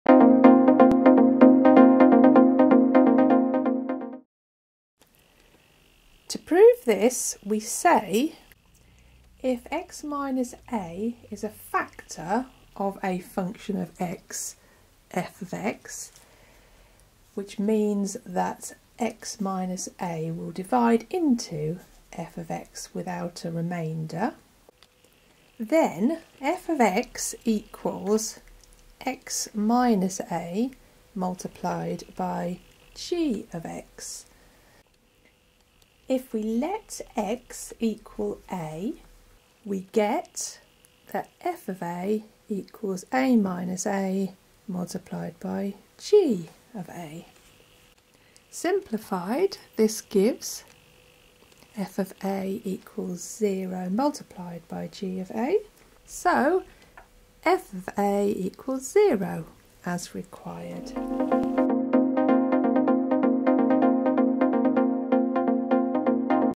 Prove that if (x-a) is a factor of the function f(x), then f(a) is zero.
This video shows how to prove that if (x-a) is a factor of the function f(x), that is if (x-a) divides into f(x) exactly with no remainder, then the value of the function at point a, f(a) is zero. This is an example of algebraic proof.